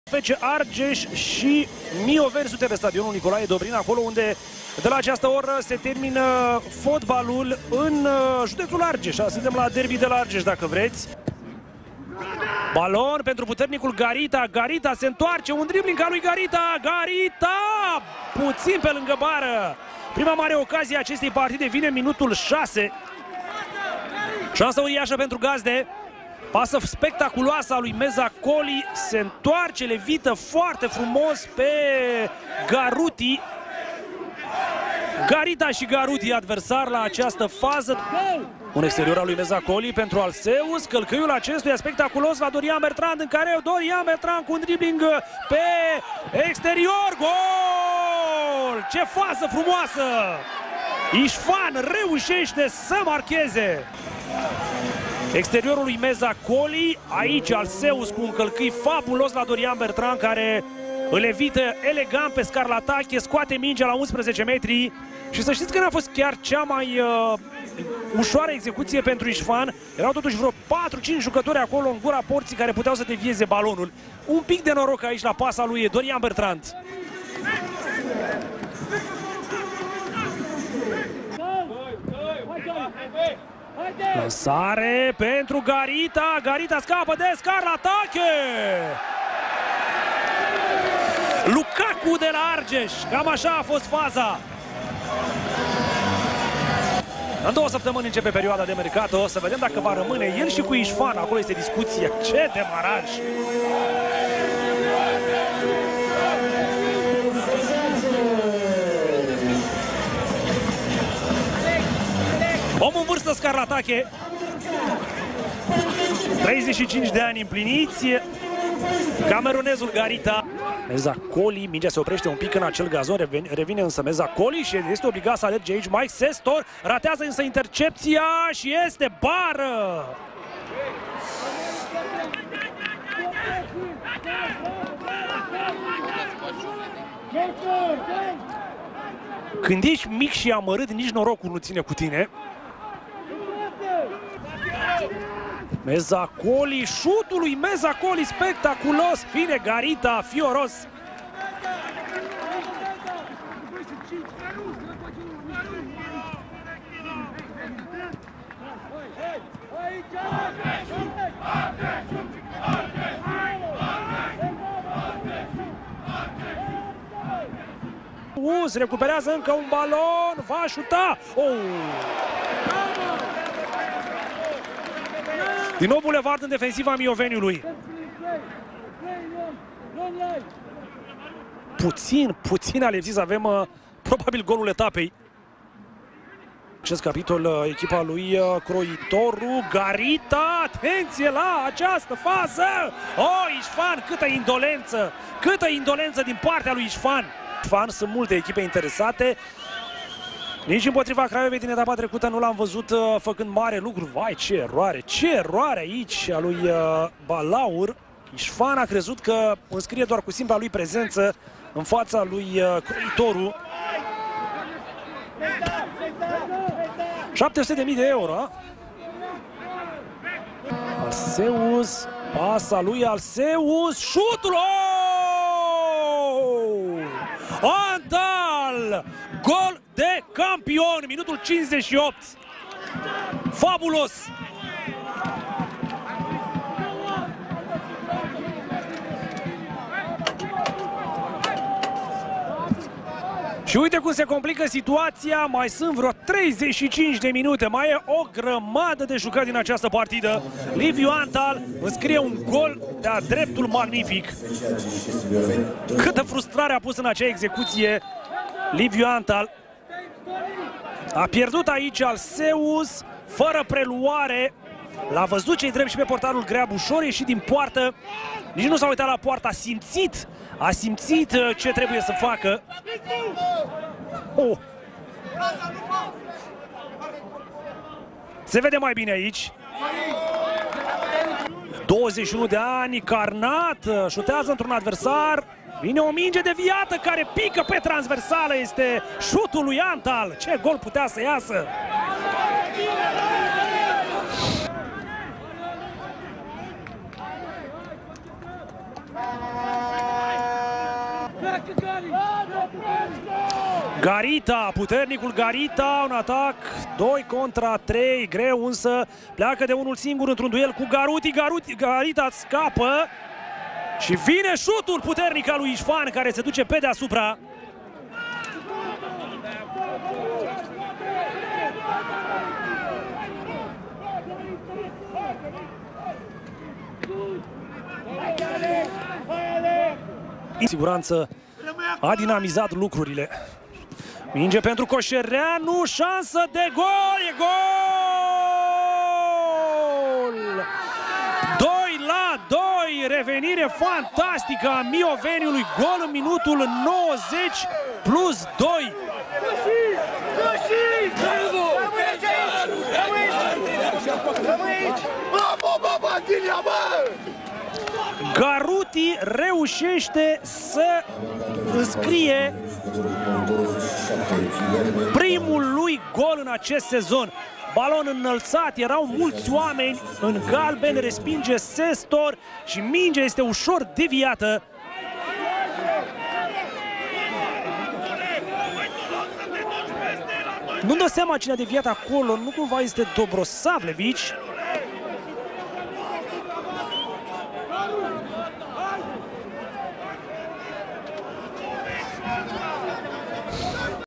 REZUMAT | FC Argeș - CS Mioveni 2-2. Super gol Liviu Antal, revenire senzațională a oaspeților
FC Argeș - CS Mioveni 2-2
Etapa 21 / Superliga
Sezonul 2022/2023
Au marcat: Ișfan 14, Garita 19 / Antal 58, Garutti 90+2

Echipele de start:
FC ARGEȘ: 12. Greab - 2. Tofan, 23. M. Constantin, 92. Cestor, 16. Vianna - 4. Meza Colli, 6. Nijke - 11. Ișfan, 21 Alceus, 24. Bertrand - 14. Garita
REZERVE: 33. Micle, 7. Rainov, 18. G. Crețu, 20. Celestine, 25. Pârvu, 27. Donisa, 70. Jakolis, 77. Covaci, 70. Jakolis, 19. A. Stoica, 17. Dobrosavlevici, 10. Calcan
ABSENȚI: I. Șerban, Straton, Turda (accidentați)
ANTRENOR: Marius Croitoru

CS MIOVENI: 13. Croitoru - 25. I. Balaur, 15. Scarlatache, 27. Garutti - 31. Carnat, 5. Lixandru, 23. Panait, 16. Rădescu, 6. Burnea - 39. Krasniqi, 8. Antal
REZERVE: 90. Burlacu, 10. Coșereanu, 17. Gutea, 99. Kabanaga, 24. V. Pop, 30. Șerbînică, 68. Sima, 7. D. Toma, R. Trif
ABSENȚI: Buziuc, Blănaru (accidentați), Oancea (suspendat)
ANTRENOR: Marius Stoica

Stadion: Nicolae Dobrin (Piteşti)
Arbitru: Rusandu Lucian (Sfântu Gheorghe) / A1: Grigoriu Mircea Mihail (Bucureşti), A2: Tunyogi Ferencz (Zalău) / Arbitru VAR: Kovacs Szabolcs (Carei), Arbitru AVAR: Bârleanu Vlad (Bucureşti)